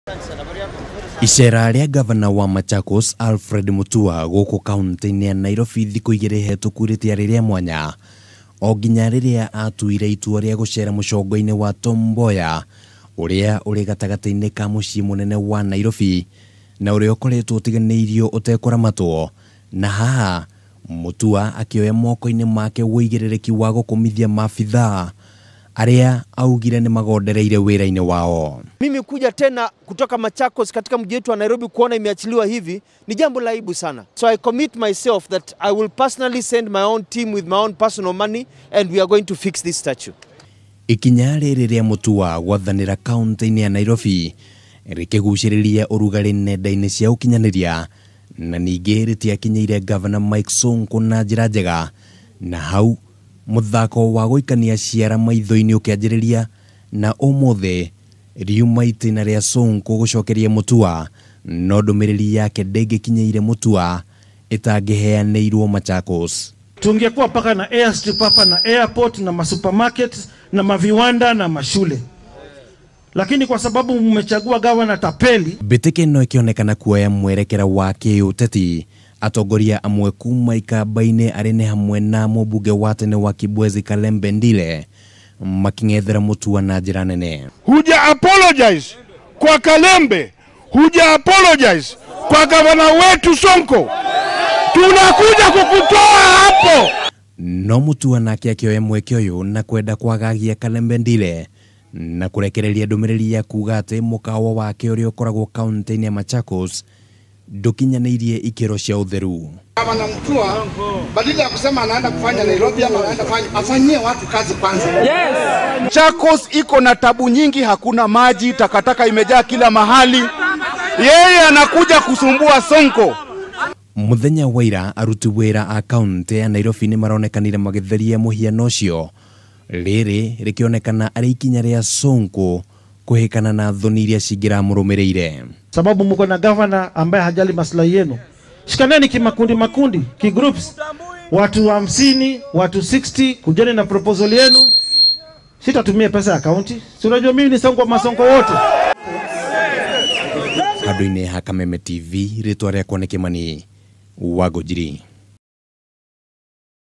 Sonko, Mutua guthimana hinya
: Ngavana aya eri kungethanira kiuteti iguru rigii mihiano